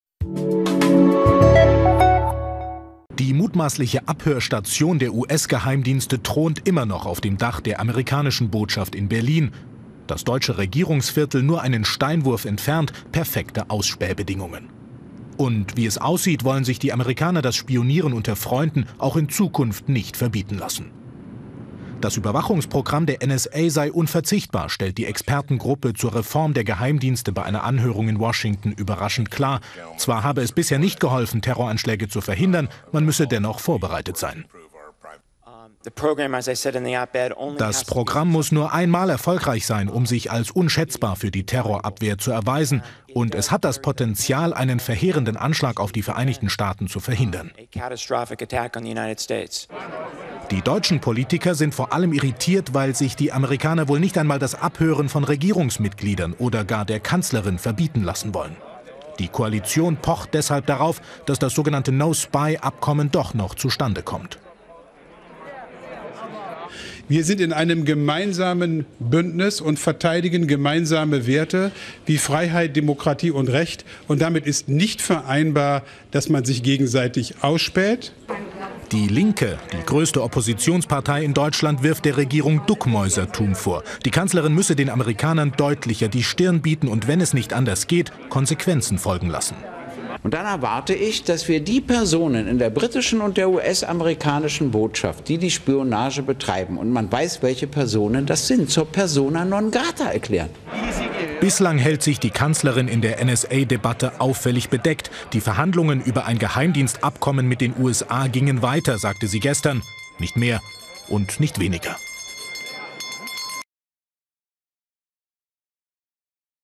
No-Spy-Abkommen vor dem Aus? | Journal
Ein bilaterales Anti-Spionageabkommen zwischen Deutschland und den USA scheint zu scheitern. Obamas Expertengruppe erklärte, dass die Methoden der NSA sehr nützlich seien und die USA vor Terroranschlägen bewahren würden. Merkels Kommentar dazu: Die Verhandlungen über ein Geheimdienstabkommen mit den USA gehen weiter.